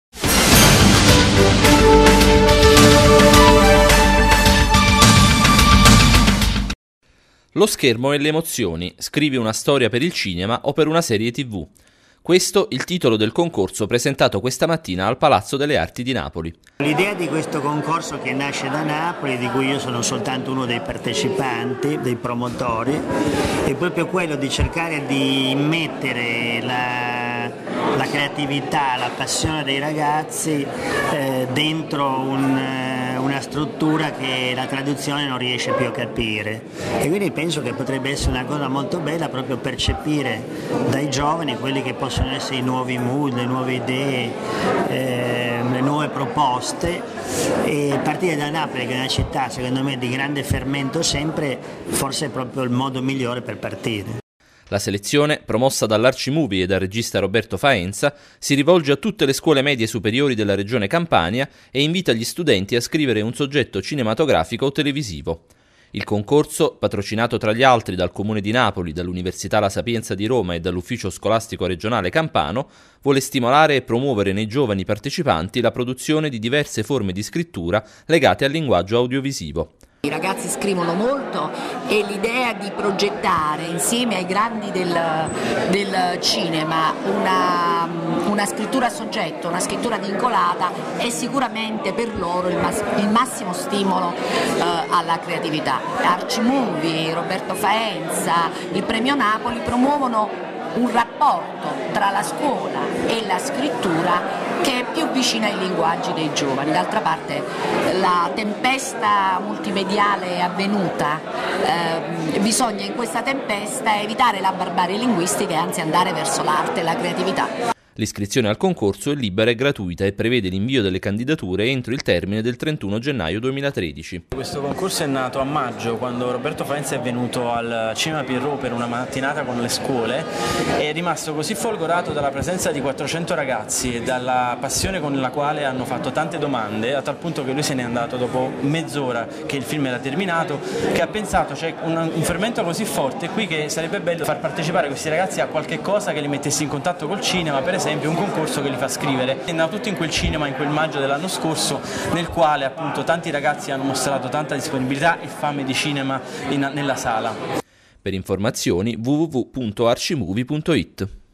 Lo Schermo e le Emozioni
LO SCHERMO E LE EMOZIONI: scrivi una storia per il cinema o per una serie tv, questo il titolo del concorso  presentato al Palazzo delle Arti di Napoli. La selezione, promossa dall' Arci Movie e dal regista Roberto Faenza, si rivolge a tutte le scuole medie superiori della Regione Campania e invita gli studenti a scrivere un soggetto cinematografico o televisivo.